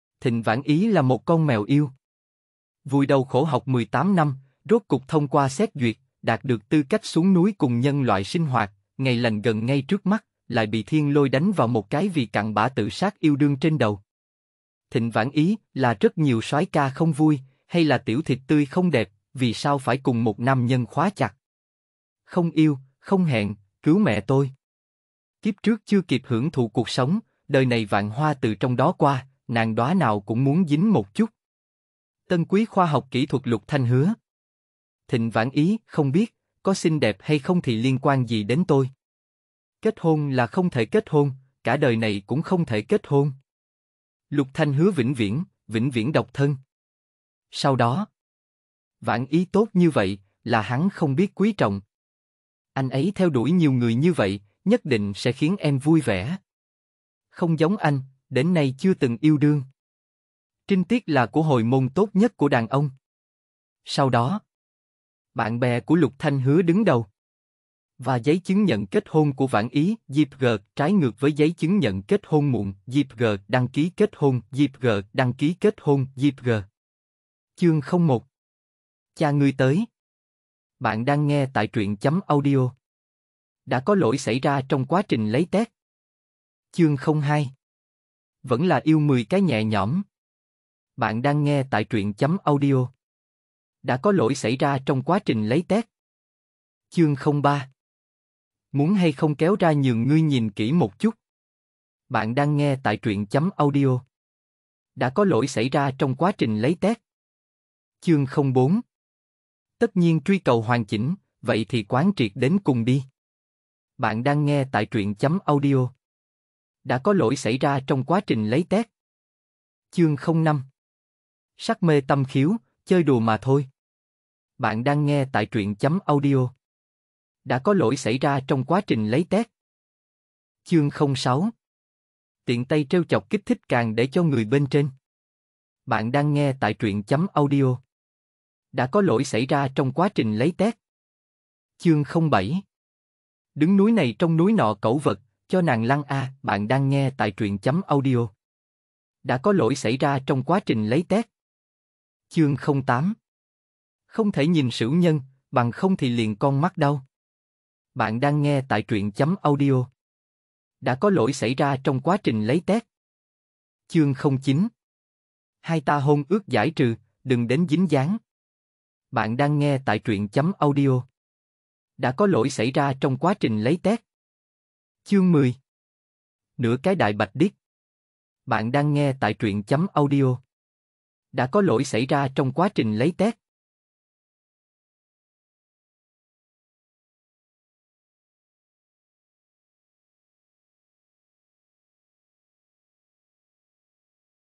Thần Toán Xuyên Thành Yêu Nhau Não, Một Quẻ Cặn Bã Nam Không Sinh Dục Chương 1-10 FULL | Lời Bài Hát
- w - đọc truyện chữ + audio

00:00:00 Chương 01: Cha ngươi tới.
00:01:26 Chương 02: Vẫn là yêu 10 cái nhẹ nhõm.
00:01:36 Chương 03: Muốn hay không kéo ra nhường ngươi nhìn kỹ một chút?
00:01:47 Chương 04: Tất nhiên truy cầu hoàn chỉnh, vậy thì quán triệt đến cùng đi.
00:01:59 Chương 05: Sắc mê tâm khiếu, chơi đùa mà thôi
00:02:10 Chương 06: , tiện tay trêu chọc kích thích càng để cho người bên trên.
00:02:21 Chương 07: Đứng núi này trông núi nọ cẩu vật, cho nàng lăn a.
00:02:31 Chương 08: Không thể nhìn sửu nhân, bằng không thì liền con mắt đau.
00:02:43 Chương 09: Hai ta hôn ước giải trừ, đừng đến dính dáng.
00:02:54 Chương 10: Nửa cái đại bạch đít.

Intro:

Thịnh Vãn Ý là một con mèo yêu.

Vùi đầu khổ học mười tám năm,

Rốt cục thông qua xét duyệt, đạt được tư cách xuống núi cùng nhân loại sinh hoạt,

Ngày lành gần ngay trước mắt, lại bị thiên lôi đánh vào một cái vì cặn bã tự sát yêu đương trên đầu.

Thịnh Vãn Ý:???

Là rất nhiều soái ca không vui, hay là tiểu thịt tươi không đẹp, vì sao phải cùng một nam nhân khóa chặt?

Không yêu, không hẹn, cứu mẹ tôi.

Kiếp trước chưa kịp hưởng thụ cuộc sống,

Đời này vạn hoa từ trong đó qua, nàng đóa nào cũng muốn dính một chút!

Tân quý khoa học kỹ thuật Lục Thanh Hứa - -

Thịnh Vãn Ý? Không biết, có xinh đẹp hay không thì liên quan gì đến tôi?

Kết hôn là không thể kết hôn, cả đời này cũng không thể kết hôn.

Lục Thanh Hứa vĩnh viễn, vĩnh viễn độc thân.

Sau đó...

Vãn Ý tốt như vậy, là hắn không biết quý trọng.

Anh ấy theo đuổi nhiều người như vậy, nhất định sẽ khiến em vui vẻ. Không giống anh, đến nay chưa từng yêu đương.

Trinh tiết là của hồi môn tốt nhất của đàn ông.

Sau đó.

Bạn bè của Lục Thanh Hứa đứng đầu:

[Và giấy chứng nhận kết hôn của Vãn Ý].jpg

[Trái ngược với giấy chứng nhận kết hôn muộn].jpg

[đăng ký kết hôn] jpg

Bạn đang nghe Thần Toán Xuyên Thành Yêu Nhau Não, Một Quẻ Cặn Bã Nam Không Sinh Dục Tập 1 chương 1 đến 10